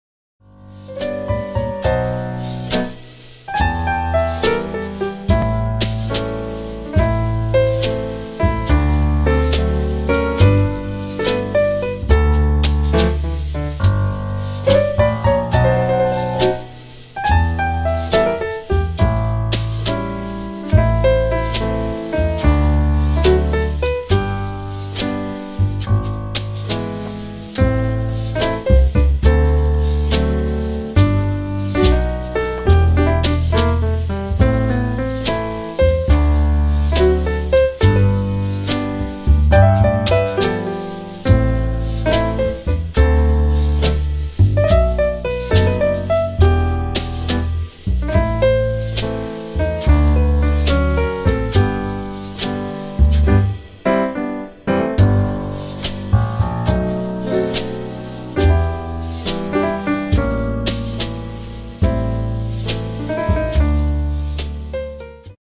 Cute shopping haul | Japanstore Hello Kitty Ceramic Coffee Dripper review ☕️
I didn’t get paid for this 😂